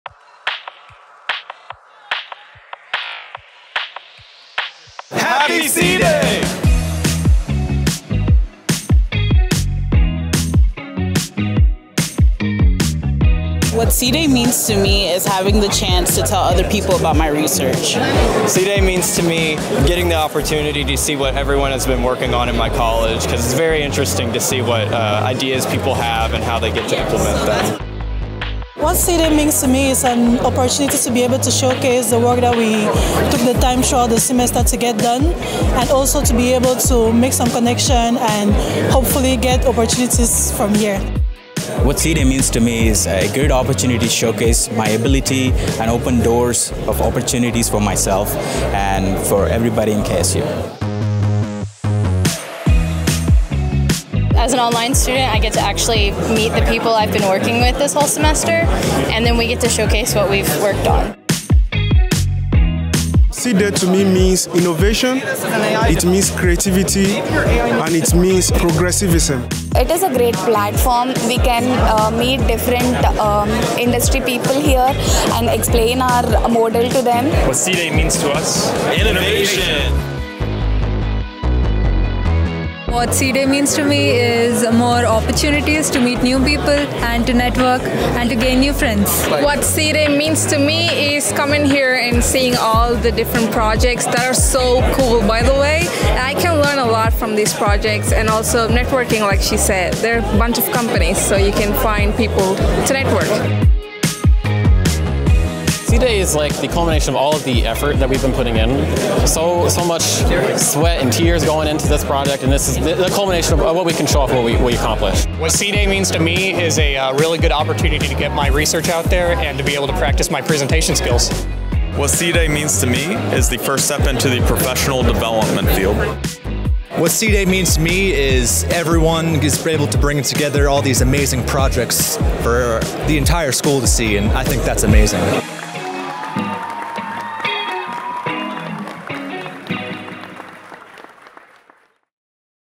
What C-Day Means to Us
Our Computing Showcase is time for our students to shine. From developing clever AI apps to creating the coolest new games, our students spend months solving thorny problems and devising novel solutions.

Take a listen to what C-Day means to our students, and huge thanks to our Fall 2024 C-Day judges, advisory board members, speakers Bruce Gibson & Jake Elsass, and sponsors The Honey Baked Ham Company & Go Studio. We're so glad to be mapping the future of computing with you by our side.